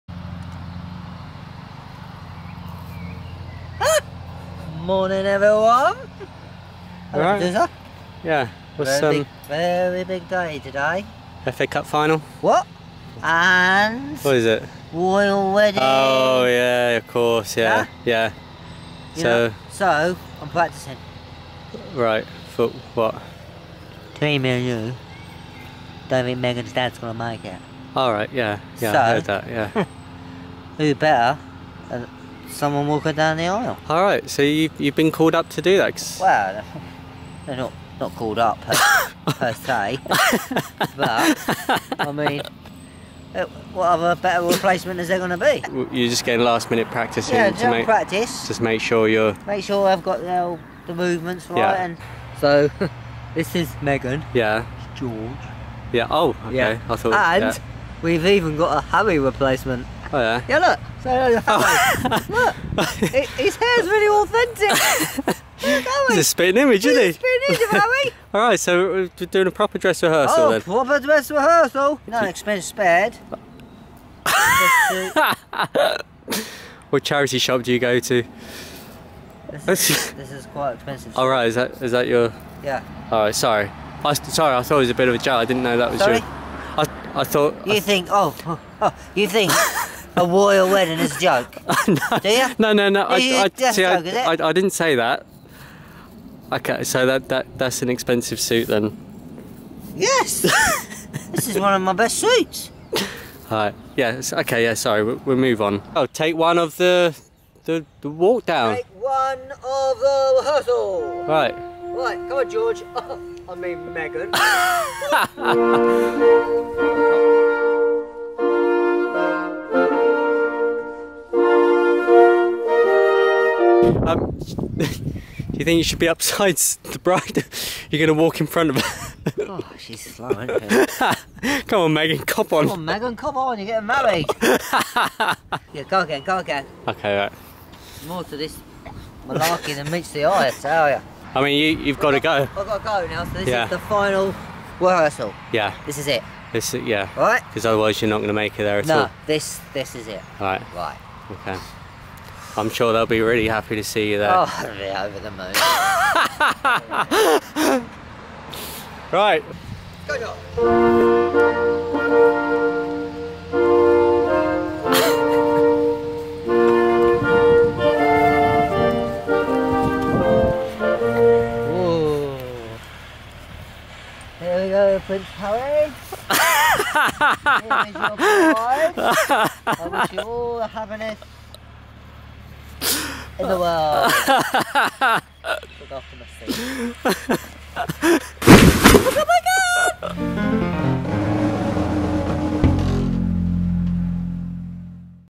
The Woyal Wedding
Who better to lead Megan down the aisle than yours twuly! just need some last minute pwactice.